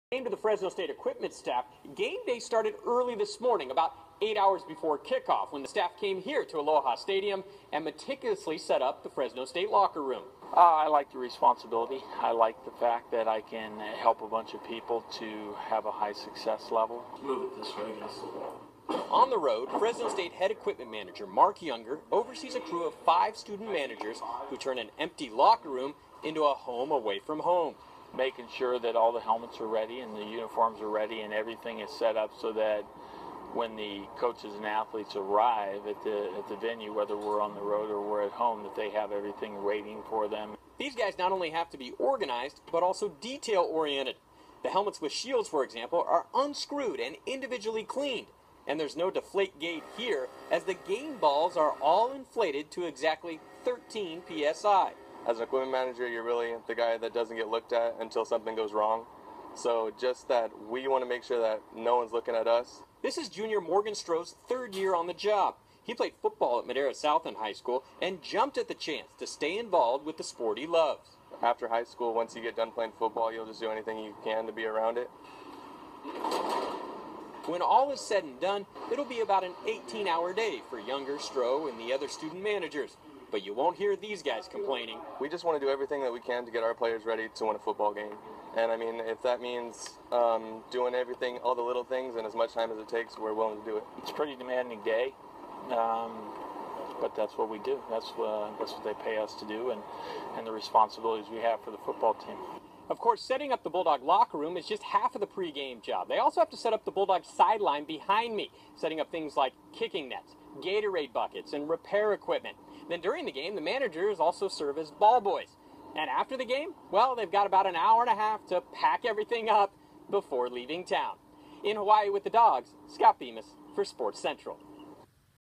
Fresno State Football Equipment Managers 2015
Fresno state equipment managers 2015 in Hawaii for KSEE 24 bulldog insider